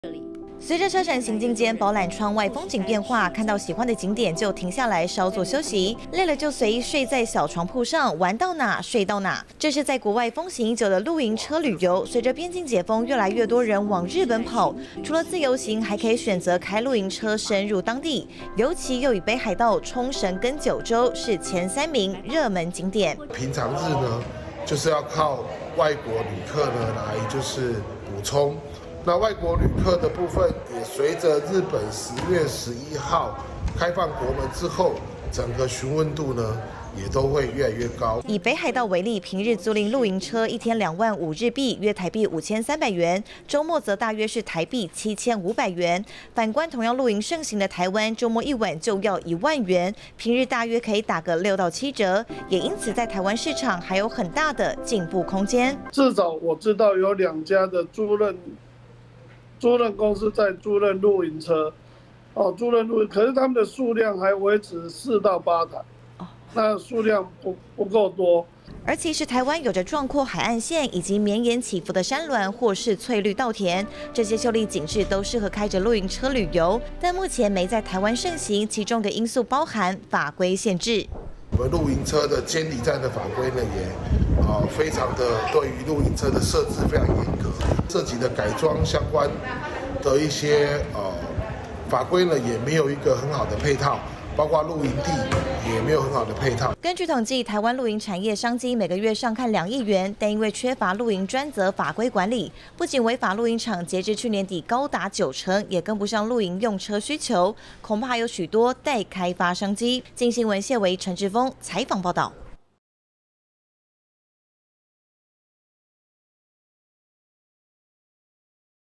近年露營風氣盛行 出國露營車旅遊正夯｜鏡新聞
在日本也可以開露營車，來趟東瀛的深度旅遊，而且當地業者的目標客群，就是台灣露營客。相較之下，雖然這幾年，台灣露營風氣盛行，但我們很少看到露營車在街上跑，主要是因為租賃車輛太少、而且法規非常嚴格。